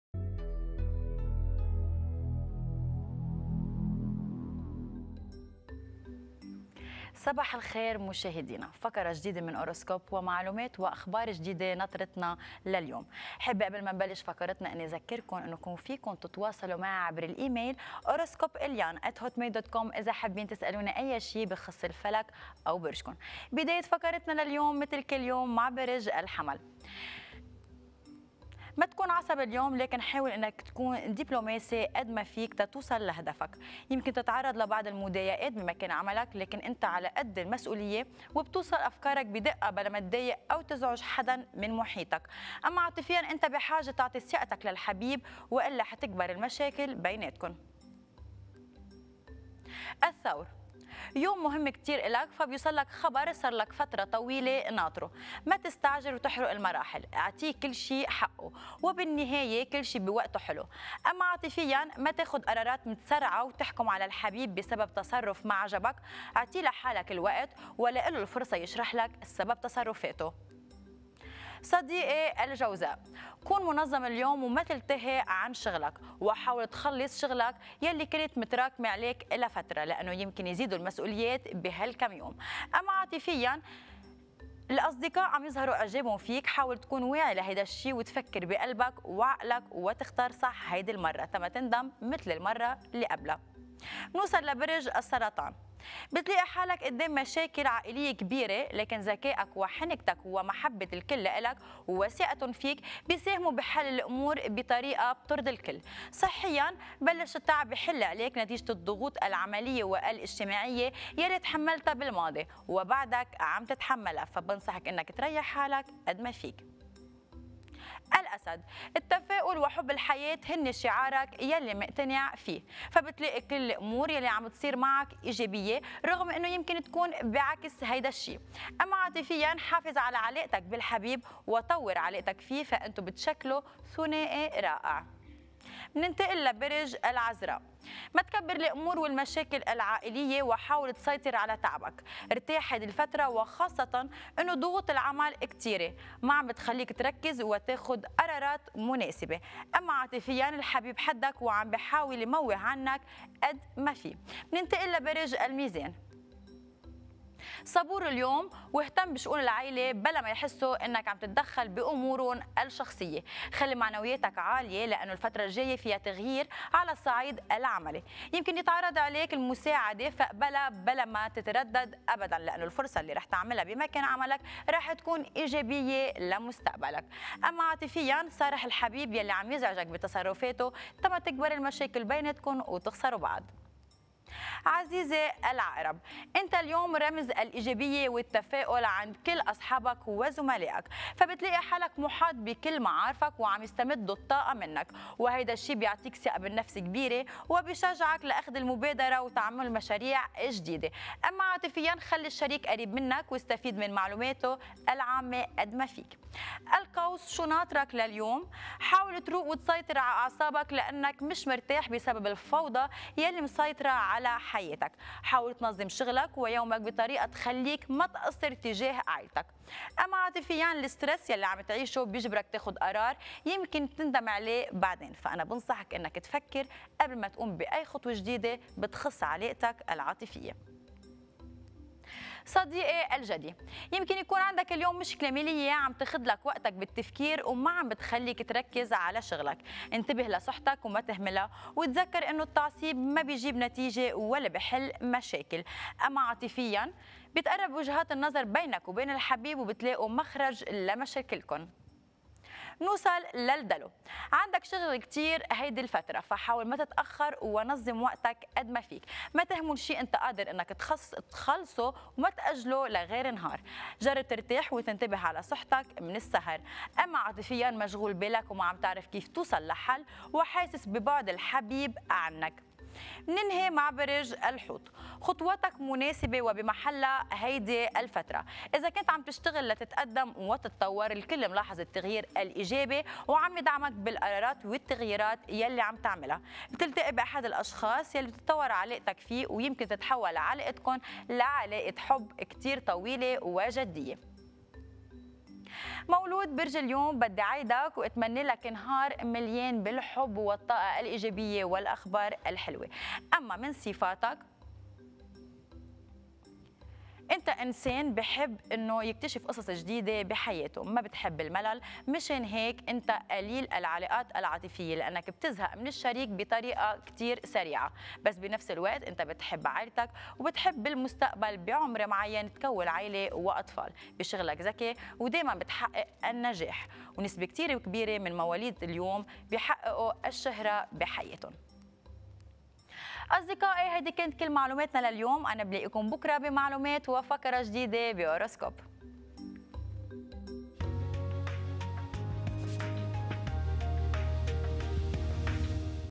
Horoscope - 12/06/2018
الابراج اليومية ومولود اليوم والشهر والتقلبات والتغييرات والتوقعات التي تهم متابعي علم الفلك، مع اليان خوند